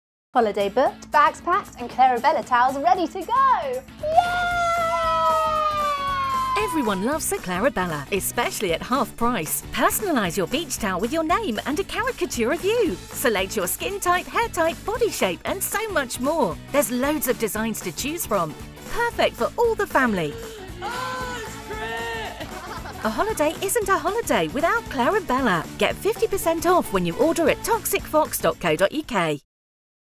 EVERYONE LOVES A CLAIREABELLA...!
Especially at half price! Personalise your beach towel with your name and a caricature of you - 50% Off when you order - TV Media Buying by ARM Direct™ - TV ad by Nick Sneath - enjoy your holidays with a Claireabella beach towel